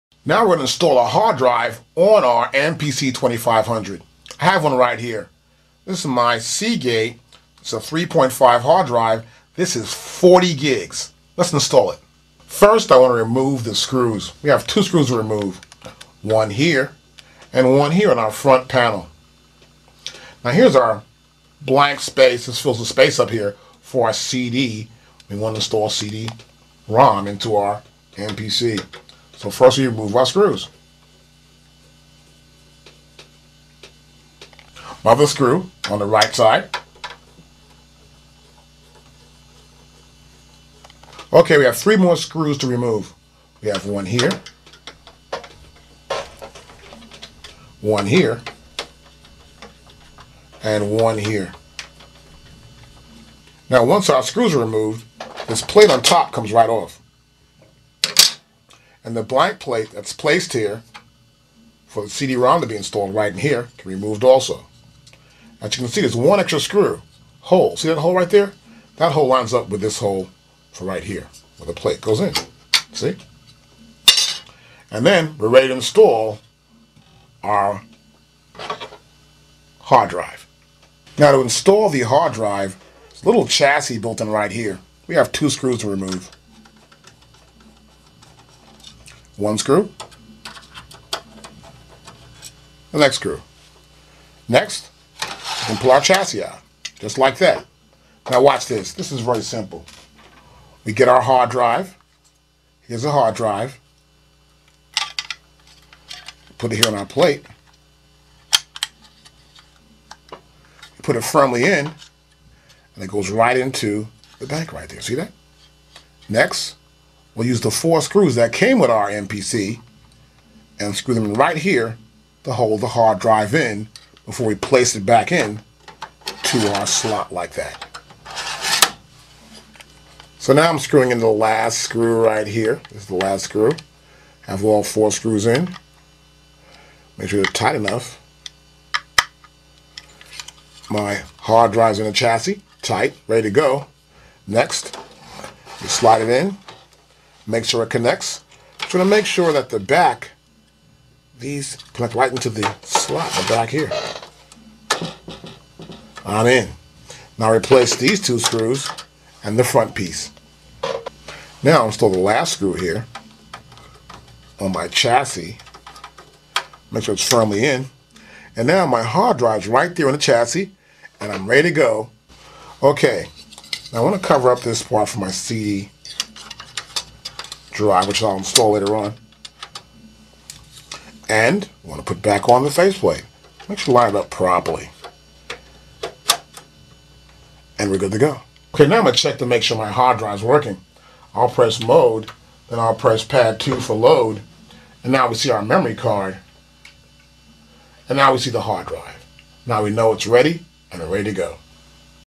Installing a Hard Drive on AKAI MPC2500
In this video we are Installing a Hard Drive on AKAI MPC2500. It's easy and simple to do just watch and your done.